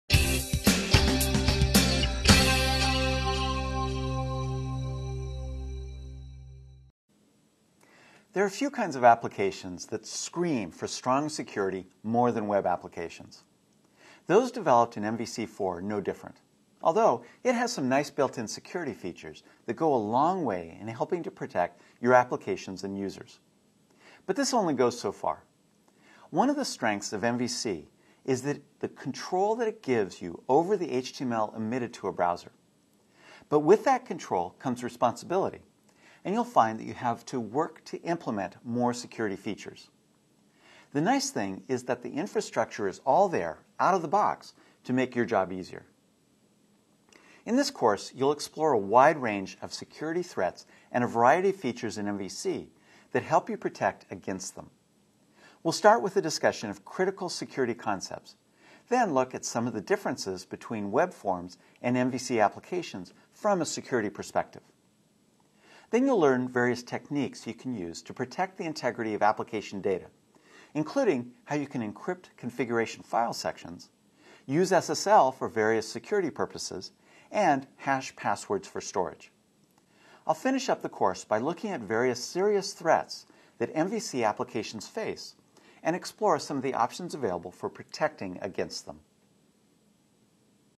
Trailer for MVC 4.0 tutorial: Security Concepts
Trailer for MVC 4.0 tutorial: "Security Concepts" with Expert Don Kiely

In this course you'll explore a wide range of security threats and a variety of features in MVC that help you protect against them. We'll start with a discussion of critical security concepts, then look at some of the differences between Web Forms and MVC applications from a security perspective. Then you'll learn various techniques you can use to protect the integrity of application data, including how you can encrypt configuration file sections, use SSL for various security purposes, and hash passwords for storage. I'll finish up the course by looking at various serious threats that MVC applications face, and explore some of the options available for protecting against them.